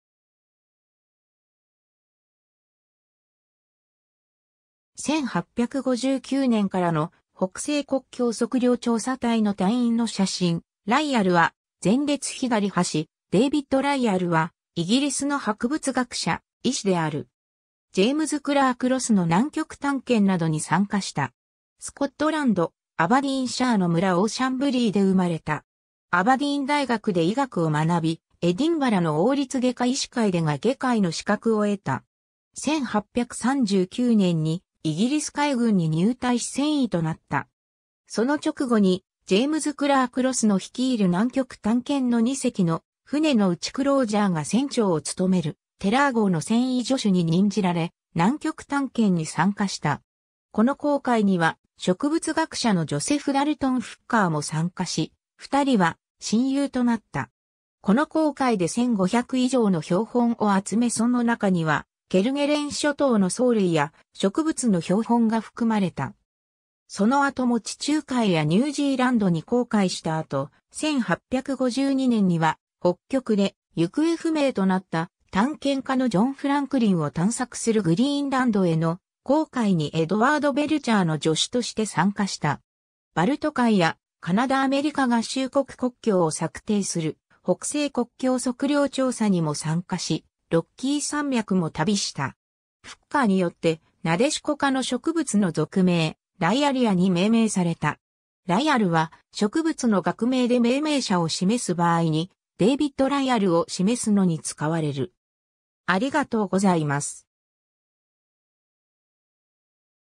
デービッド・ライアル
1859年からの北西国境測量調査隊の隊員の写真、ライアルは前列左端 デービッド・ライアル（David Lyall、1817年6月1日 - 1895年3月2日）はイギリスの博物学者、医師である。
ジェイムズ・クラーク・ロスの南極探検などに参加した。
 スコットランド、アバディーンシャーの村Auchenblaeで生まれた。
アバディーン大学で医学を学び、エディンバラの王立外科医師会でが外科医の資格を得た。
1839年にイギリス海軍に入隊し船医となった。
その直後にジェームズ・クラーク・ロスの率いる南極探検の2隻の船のうちクロージャー（Francis Crozier）が船長を務めるテラー号の船医助手に任じられ、南極探検に参加した。
この航海には植物学者のジョセフ・ダルトン・フッカーも参加し、2人は親友となった。
この航海で1500以上の標本を集めその中にはケルゲレン諸島の藻類や植物の標本が含まれた。
 その後も地中海やニュージーランドに航海した後、1852年には北極で行方不明となった探検家のジョン・フランクリンを探索するグリーンランドへの航海にエドワード・ベルチャー（Edward Belcher）の助手として参加した。
バルト海やカナダ＝アメリカ合衆国国境を策定する北西国境測量調査にも参加し、ロッキー山脈も旅した。
 フッカーによってナデシコ科の植物の属名 Lyalliaに命名された。
 Lyallは、植物の学名で命名者を示す場合にデービッド・ライアルを示すのに使われる。